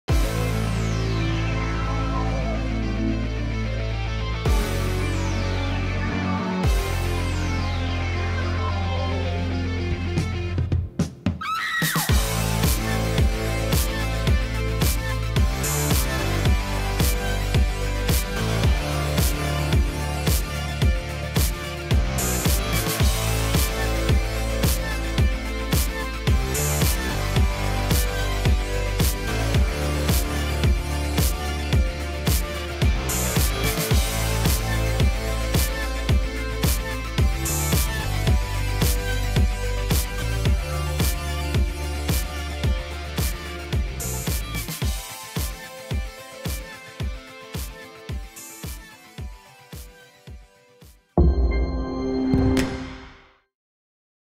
Är du den fräcka typen?
Upplev natten med din partner - Nya MICRA. Instegsbelysning och en magnetisk smartphone-hållare gör natten till din. Välj en ny designlampa från Nissan för att skapa en riktigt speciell stämning vartän du vill.